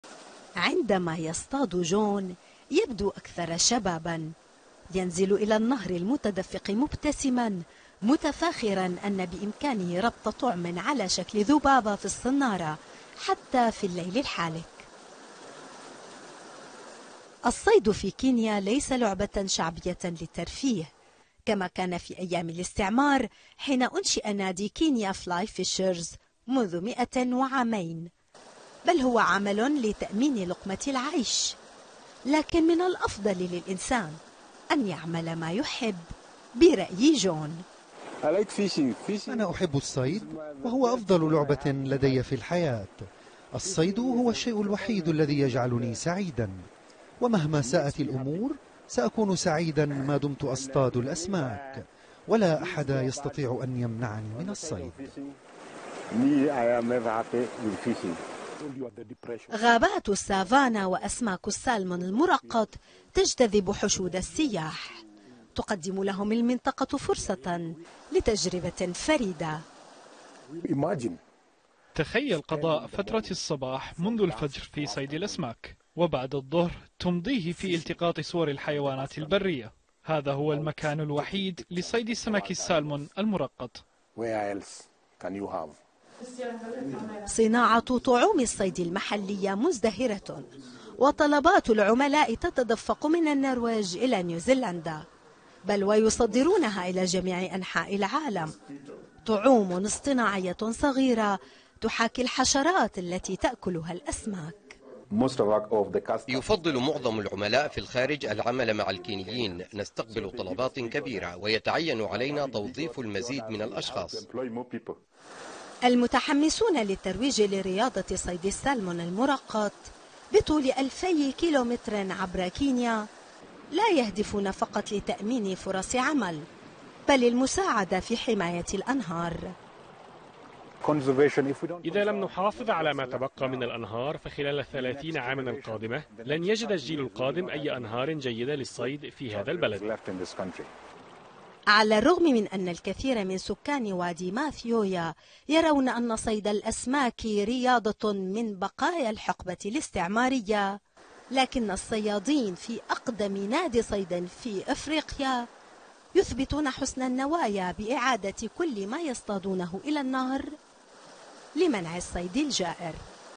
الغاية تبرر الوسيلة.. كيف تحول صيد الأسماك في غابات السافانا في كينيا إلى وسيلة لجذب السياح؟
لا يصطاد الكينيون من أجل المتعة بل لأكل الأسماك، لكن إن كانت رياضة الصيد ستضمن دخلا وتجلب السياح لحماية الأنهار، فإن الغاية تبرر الوسيلة.
تقرير: نسرين طرابلسي